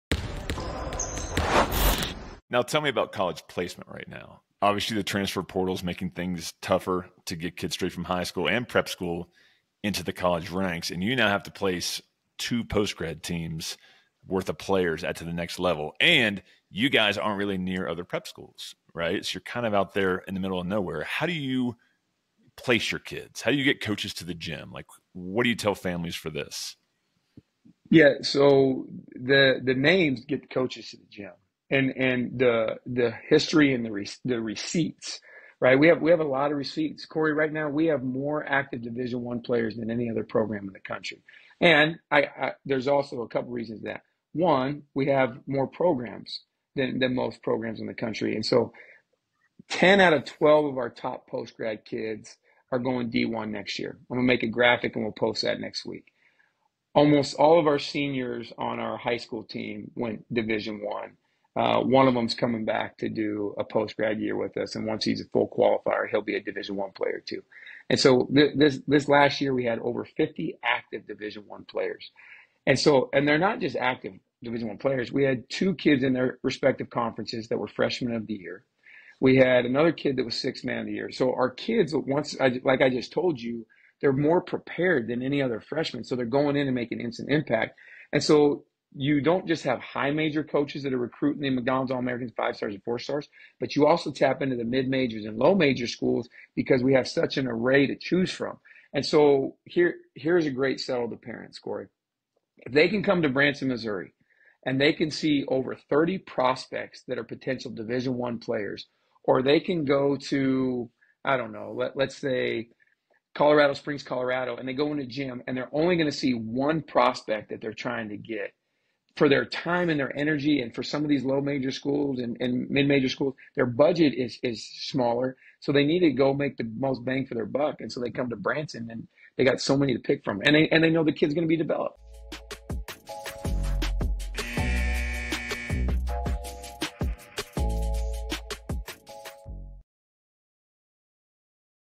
Why College Coaches Love Visiting Link Year feat. Adam Donyes, Founder of Link Year Prep
In this clip from the PREP Athletics Podcast, we delve into why college coaches flock to Link Year Prep to scout their top-tier talent. Adam Donyes, founder of Link Year, discusses the unique advantages that make Link Year a hotspot for college recruiters. Discover how this program consistently produces Division I players and why it's a preferred destination for coaches across the country.

📌 Key Points:

🏀 Top Talent
Learn about the impressive track record of Link Year in producing Division I players.

💪 Preparedness
Understand how Link Year athletes are more prepared to make an immediate impact in college.

🌟 Recruiting Advantage
Find out why Link Year’s location and program structure are beneficial for both coaches and players.

💰 Economic Efficiency
See how coaches maximize their recruiting efforts by visiting Link Year.